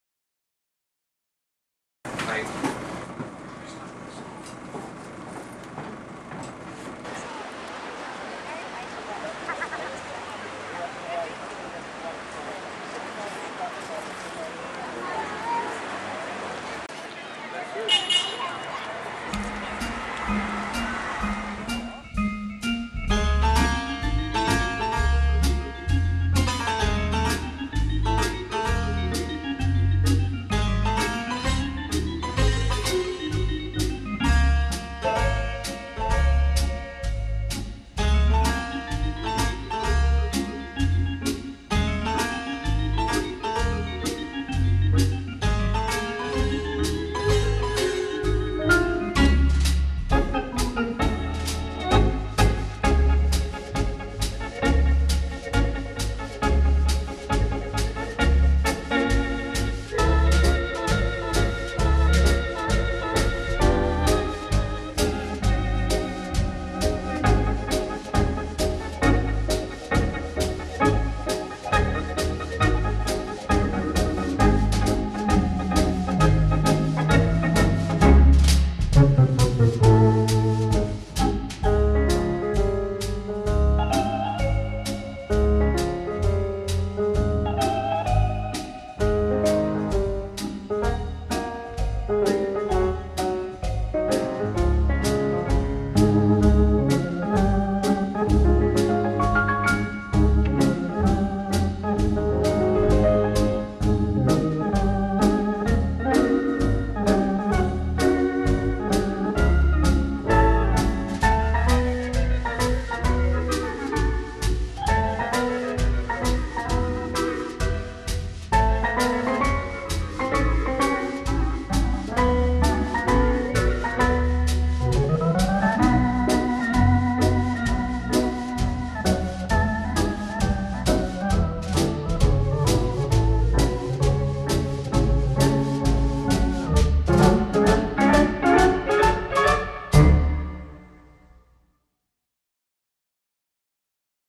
Eastbourne Pier
A brief look at Eastbourne Seafront and Pier including the Camera Obscura